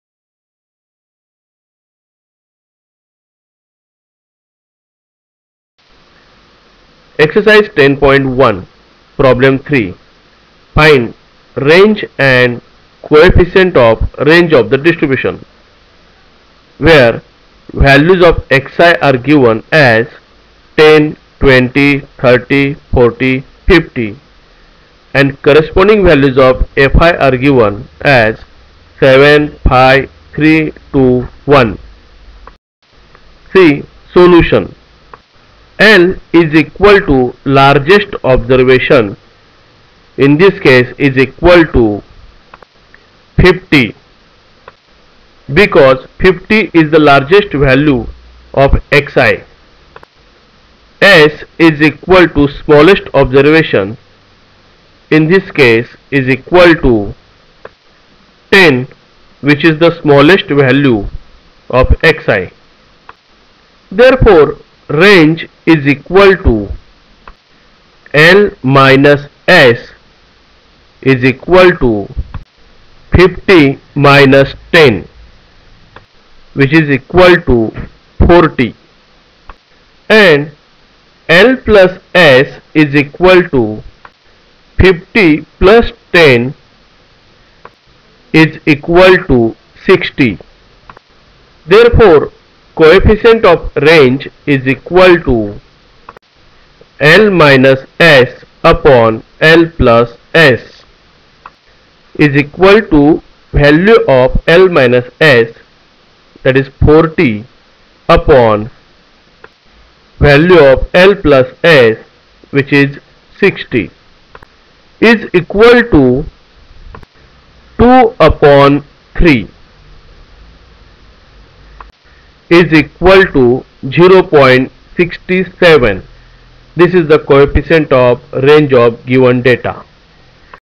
3. STATISTICS Find (i) Range and (ii) co-efficient of Range of given distribution of x and f
MATHS : STATISTICS:   Measures of Dispersion RANGE and it's Problems